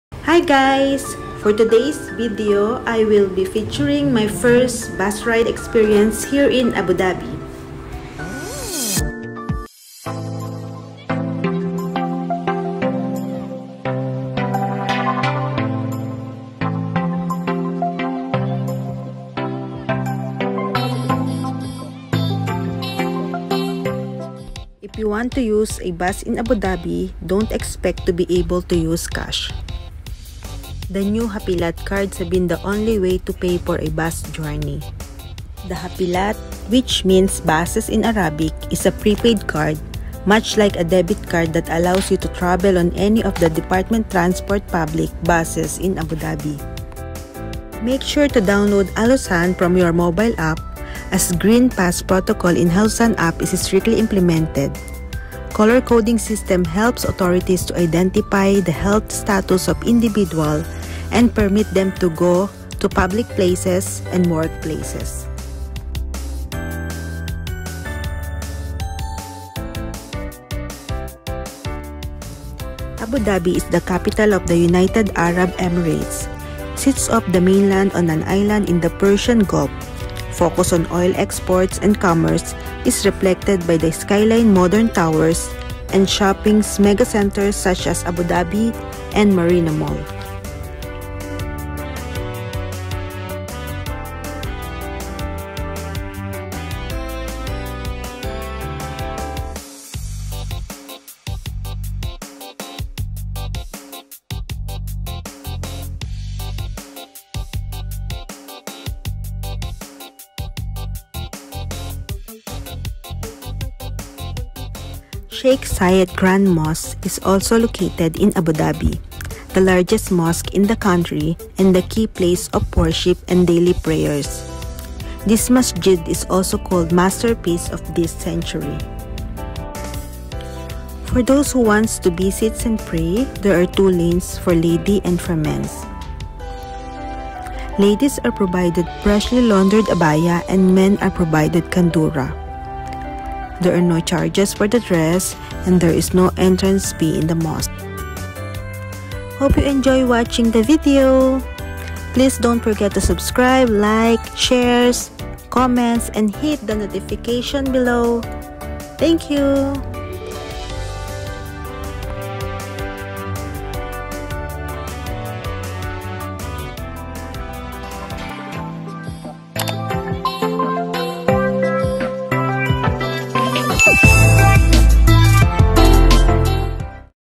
Abu dhabi Bus Ride Experience
It’s my first time experience riding bus in abu dhabi. For those who haven’t tried yet make sure you have hafilat card and download alhosn app.

Please do not download or re-upload my video